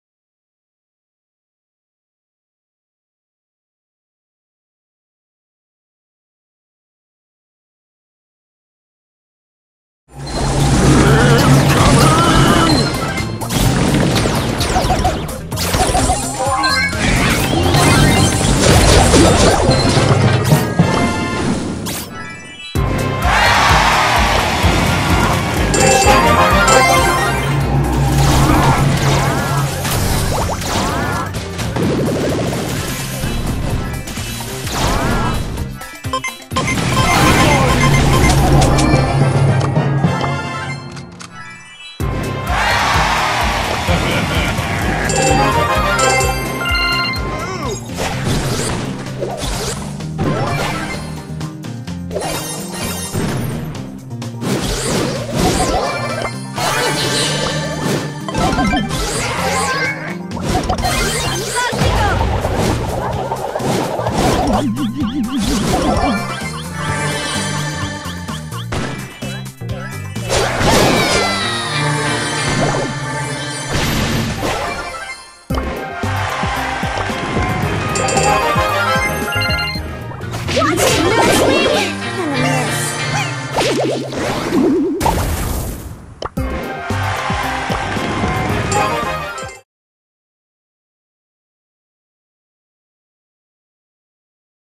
The Start of the Road to 35k
Today, we start the road to 35k by getting a bunch of brawlers ranked up.